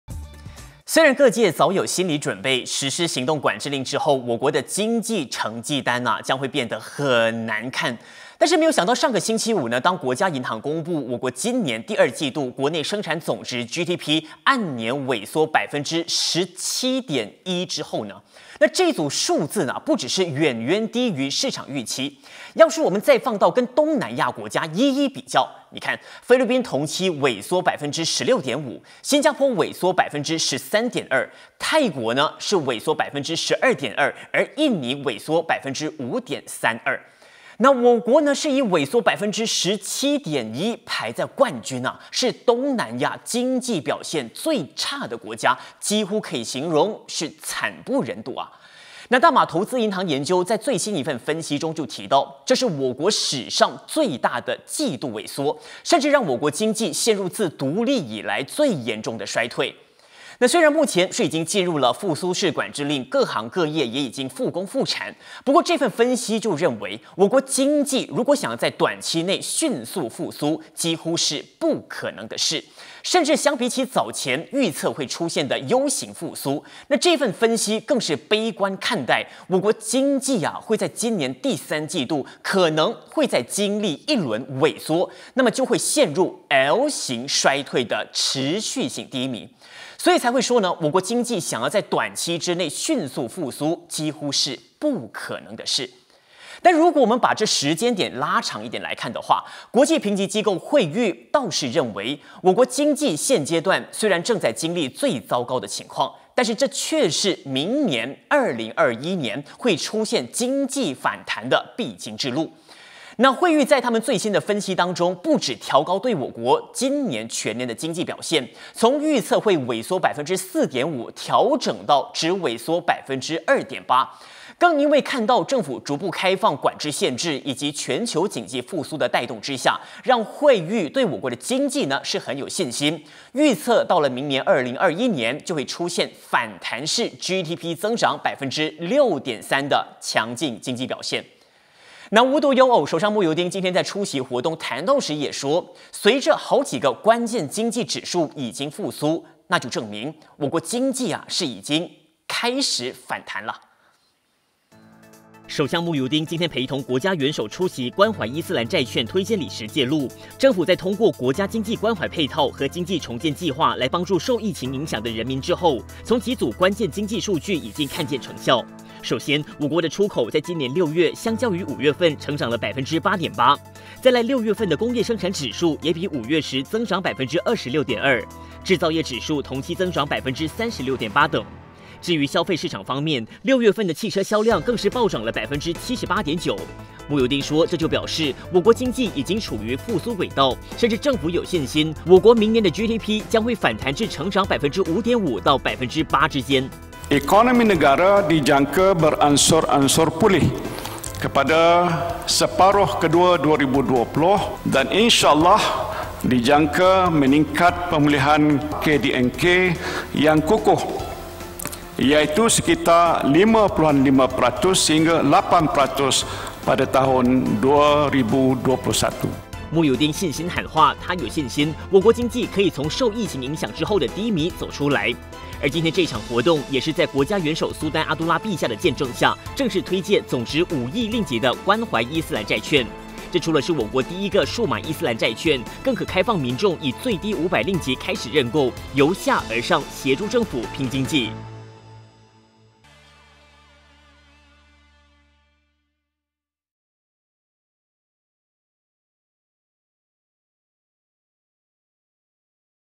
八点最热报 18/08/2020 第三季或再萎缩 分析: 我国经济陷L型衰退
大马投资银行研究有一份研究指出，2020年第二季GDP萎缩17.1%创新低后，他们预测今年第三季恐会继续萎缩，让我国经济呈“L型衰退”持续低迷，要在短期内复苏很难！